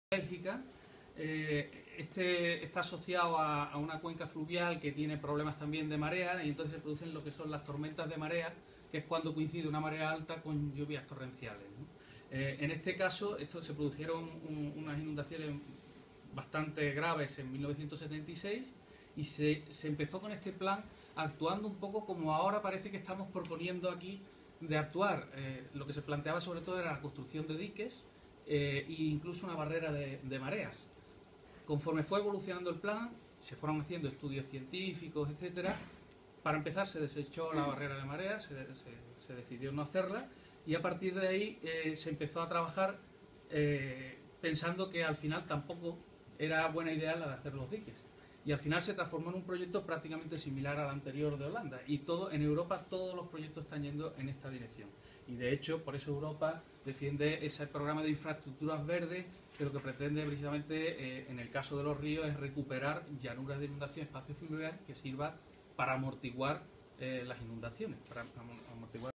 3ª Parte de Intervención de Tony Herrera.
Intervención de Tony Herrera en la presentación en rueda de prensa de ECODES, presenta proyectos de restauración fluvial asociados a gestión de inundaciones. Ejemplo de Plan Sigma, Bélgica. Más información